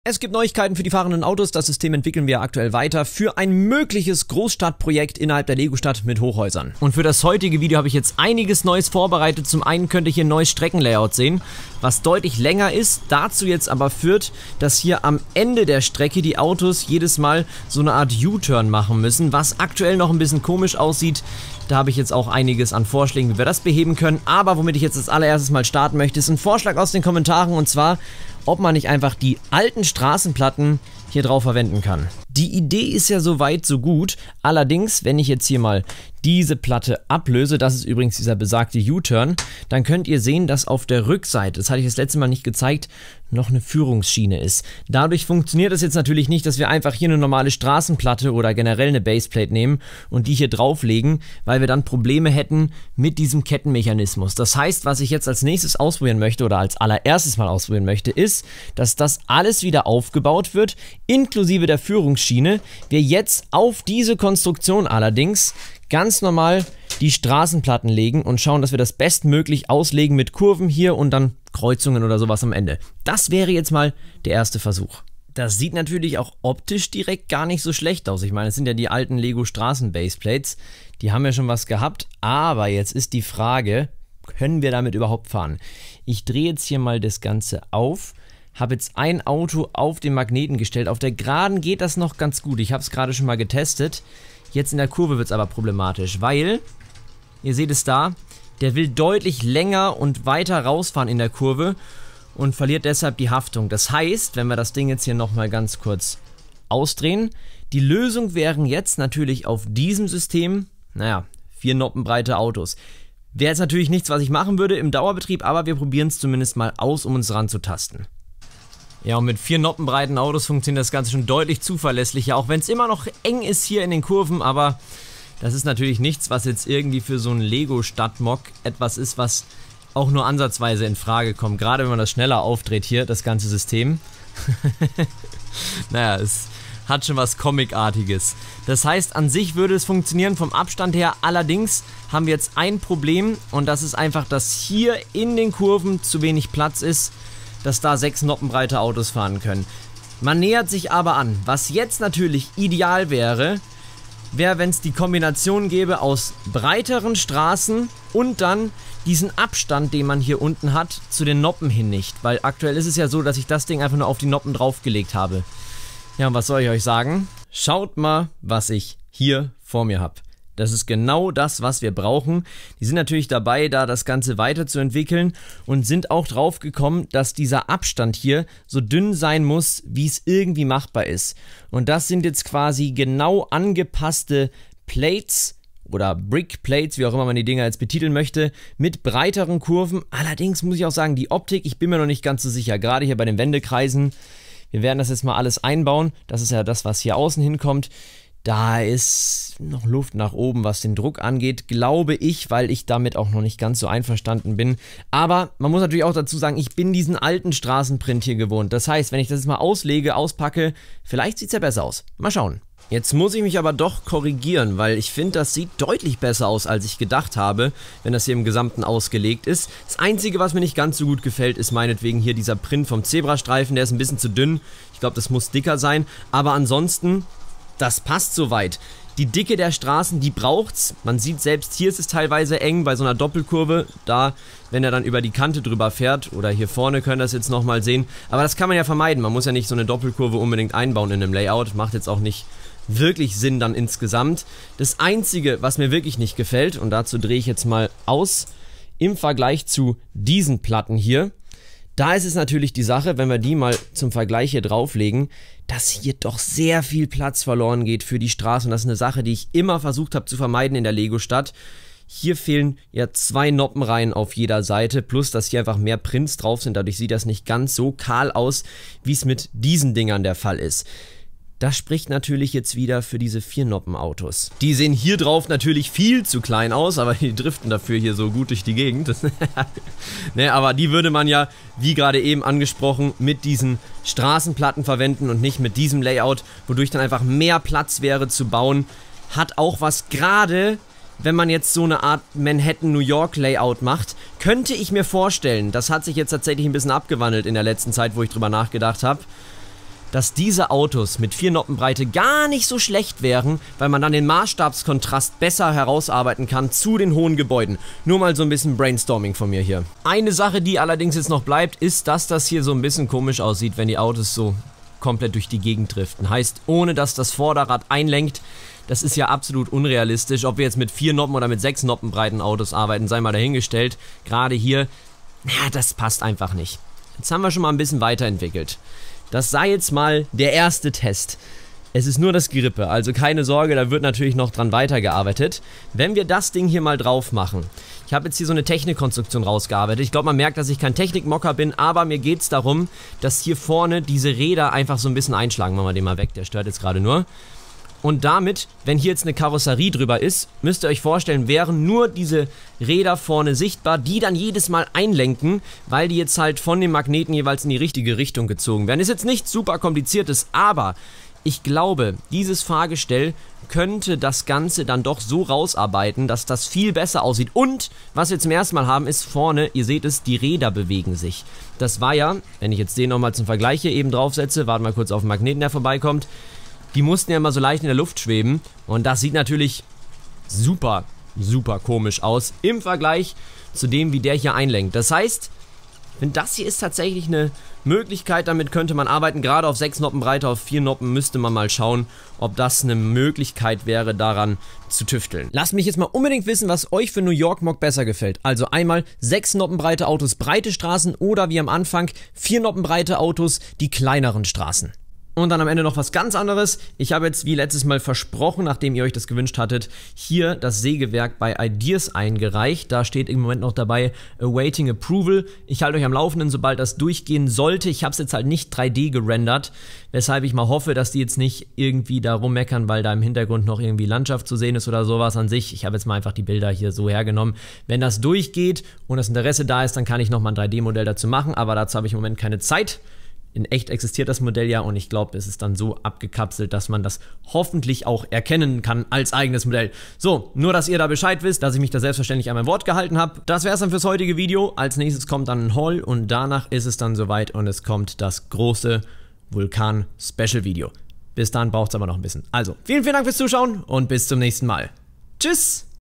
Update zu den fahrenden LEGO Autos... (und eventuell ein New York MOC?)
Neuigkeiten zu den Fahrenden Lego Autos!!